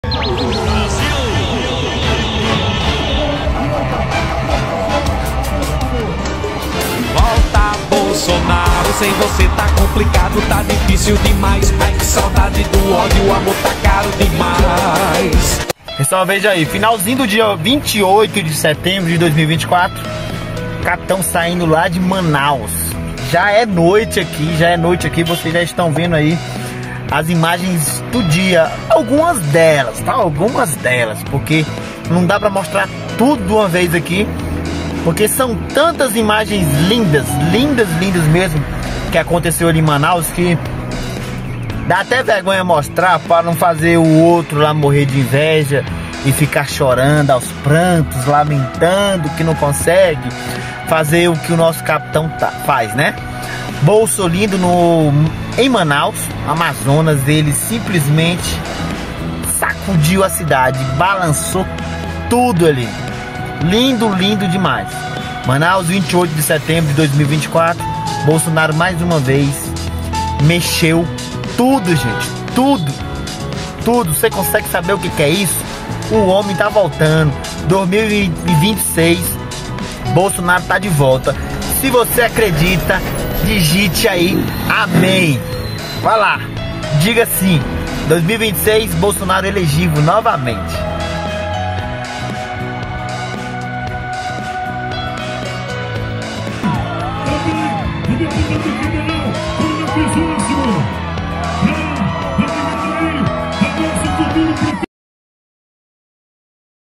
Bolsolindo assim em Manaus 28/09/24
MANAUS TEVE O GOSTINHO REVEBER O EX MAUS AMADO DO BRASIL HOJE 28/09/24

Primeiramente que Deus vos abençoe grandemente e que a paz de Deus seja abundantemente na sua vida.

🇧🇷👉 PESSOAL QUEM QUISER ME SIGA NO INSTAGRAM.JUNTOS SOMOS MAIS FORTES